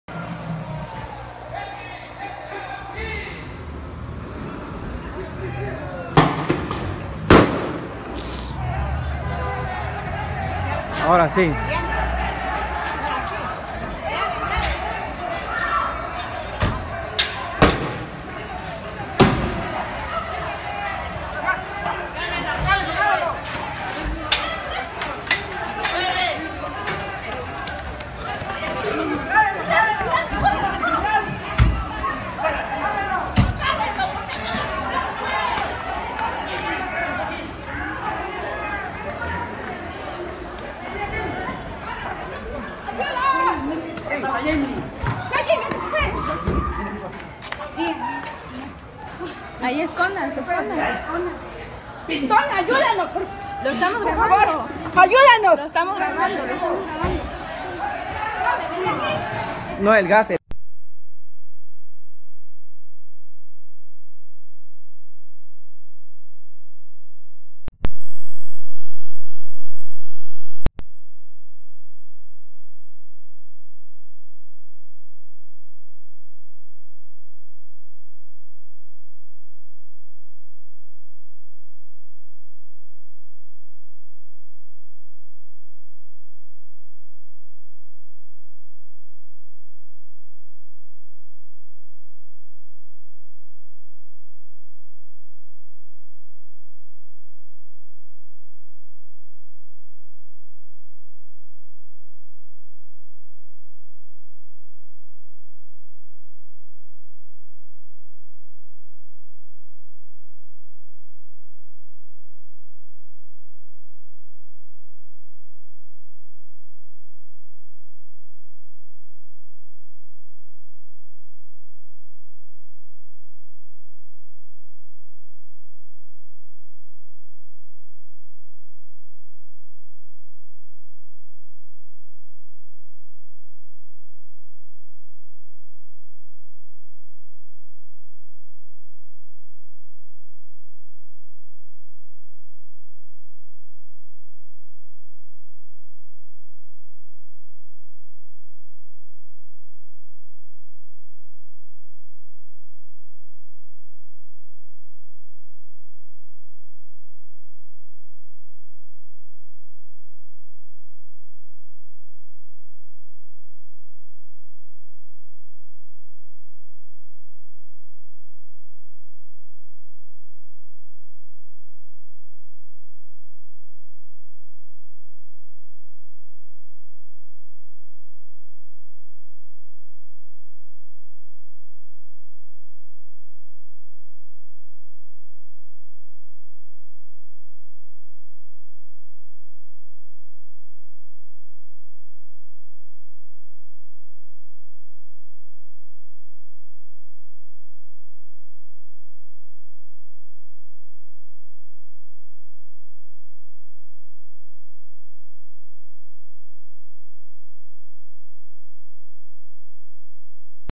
Enfrentamiento en el centro de Tuxtla
Esta noche se registró un enfrentamiento entre policías y civiles en las inmediaciones de la 1a sur y 5a poniente de la capital de Chiapas.

Versiones aseguran que el conflicto inició por un grupo proveniente de San Juan Chamula que arremetió contra una financiera que los defraudó; grupos antimotines tomaron el control de la zona.